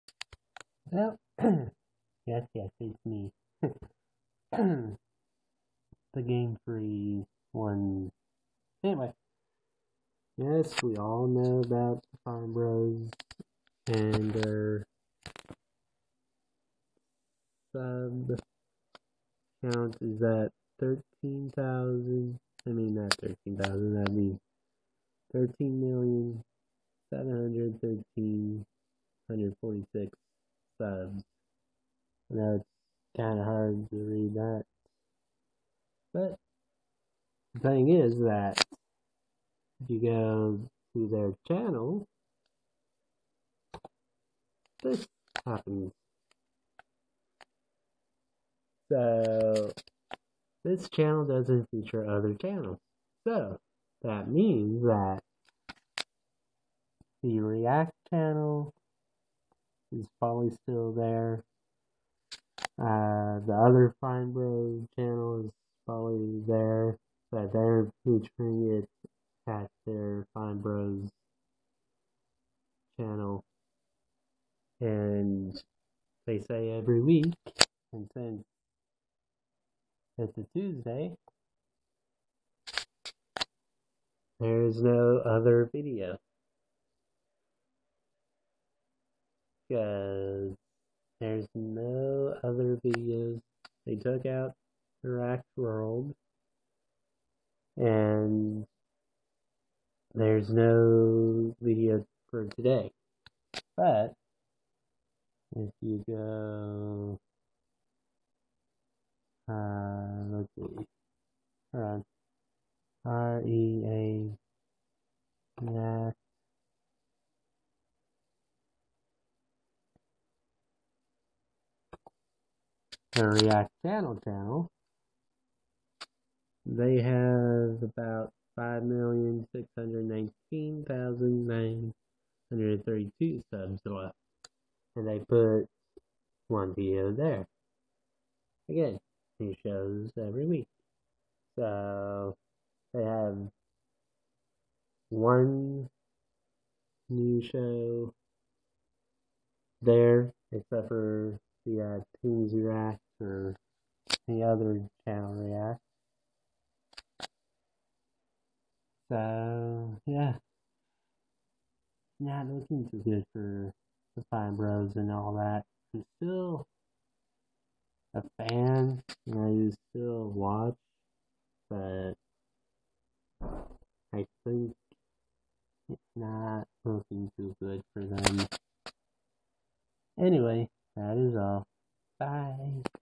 Are fine brothers done
Are the fine brothers done? They have not done a video for today or anything.